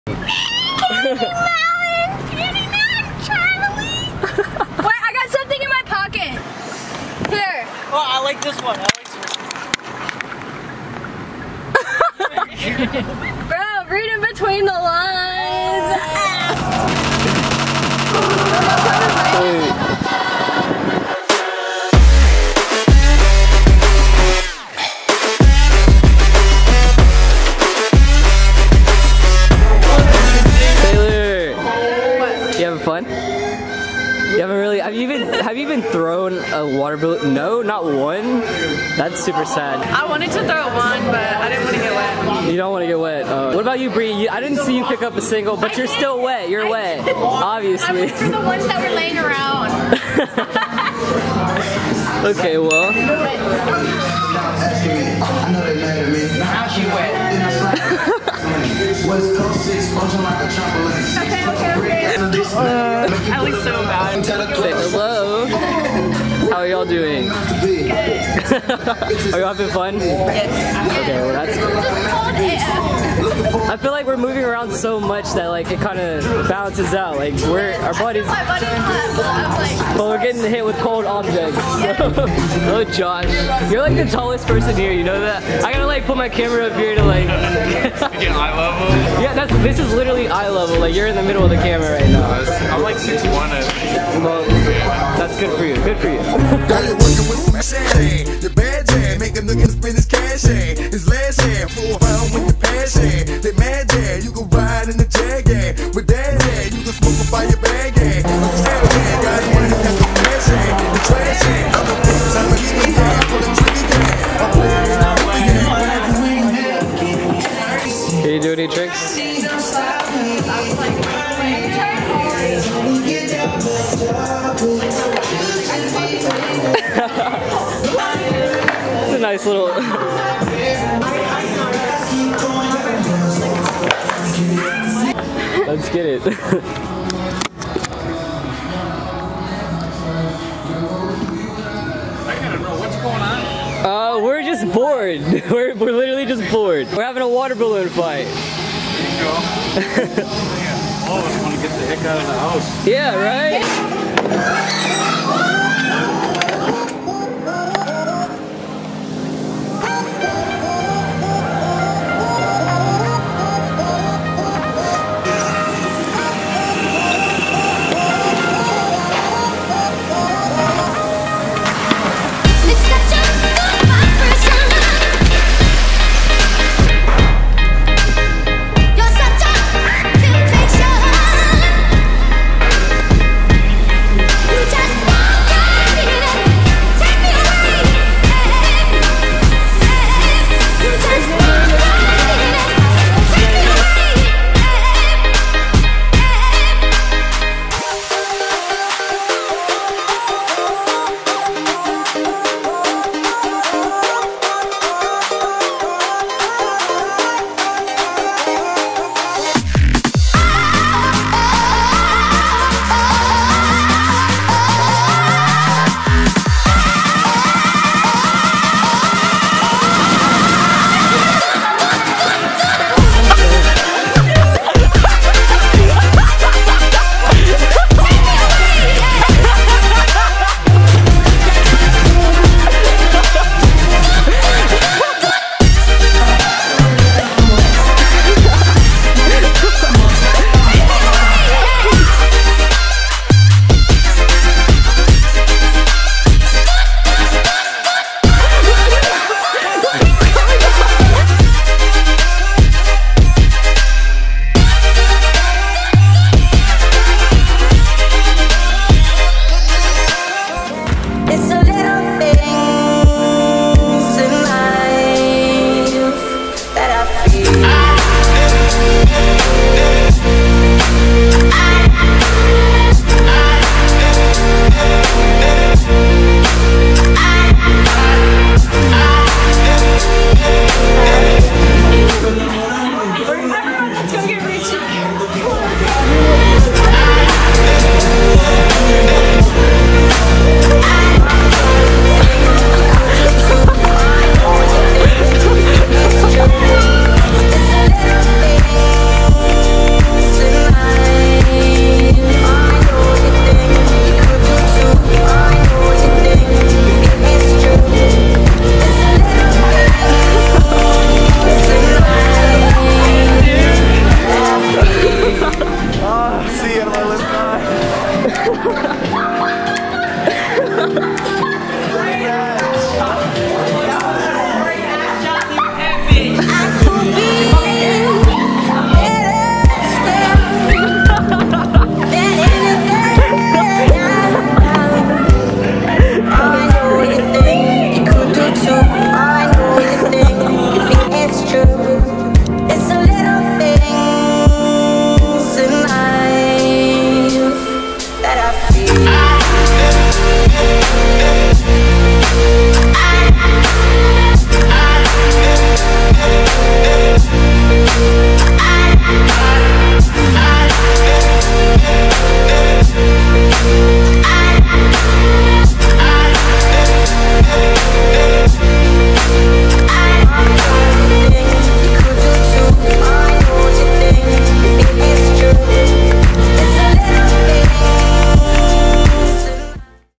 EXTREME WATER BALLOON TOURNAMENT | Lexi Brumback
Once the shelter in place lifted, my friends and I decided to have an epic water balloon fight because we were itching to get out again. We bought hundreds of balloons, took over a park, and had a free-for-all war. No rules, each man for himself. I hope you enjoy the behind the scenes of my friends and I having fun!